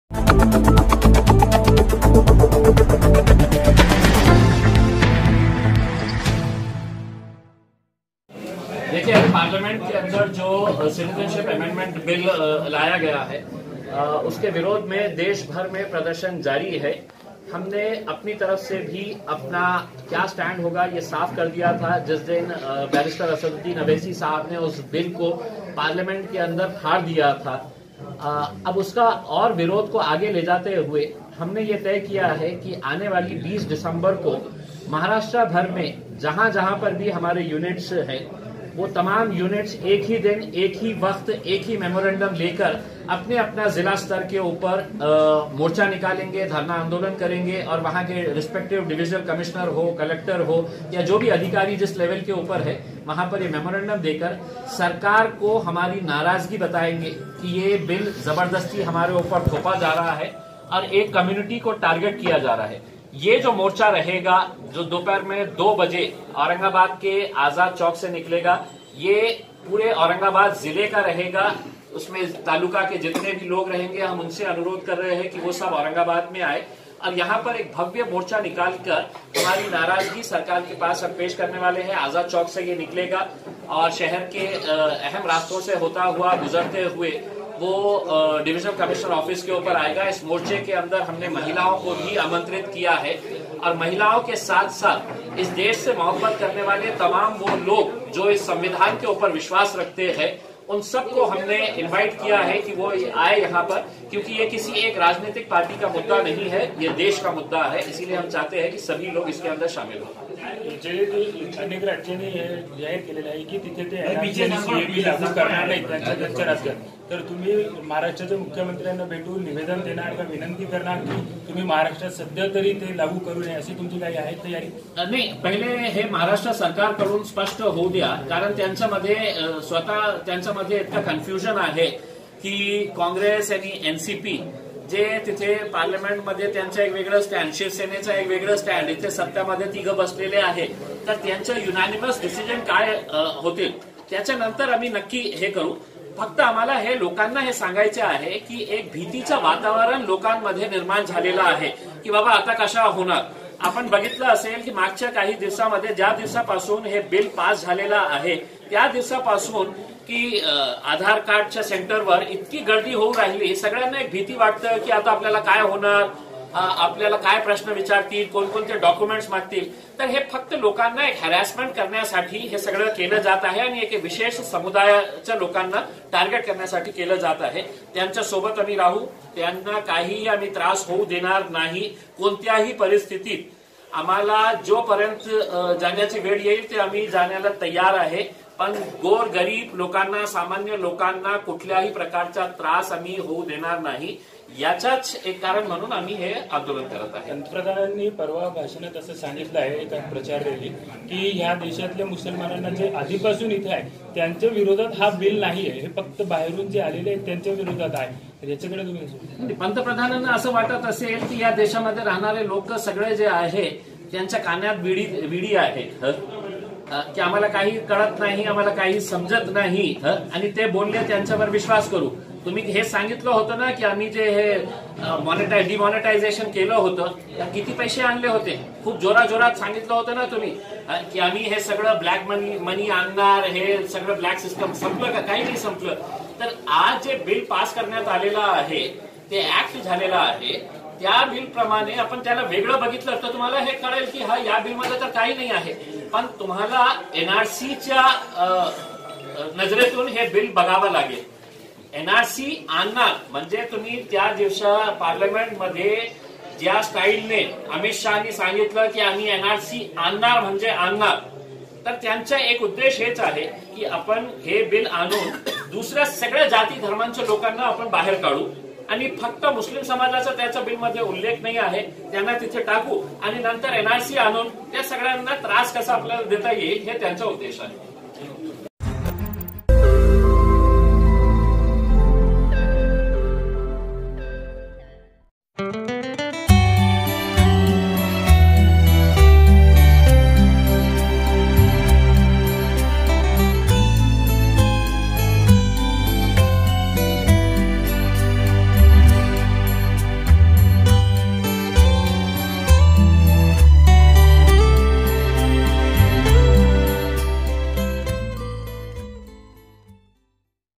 औरंगाबाद मे CAB NRCके खिलाफ आज़ाद चौक से महा मोर्चा कौम के मसले पर एक जुट होने MPइमतियाज जलील की अपिल
20 दिसंबर को औरंगाबाद मे CAB NRC के खिलाफ को आज़ाद चौक से महा मोर्चा कौम के मसले पर एक जुट होने की M.P इमतियाज जलील की अपिल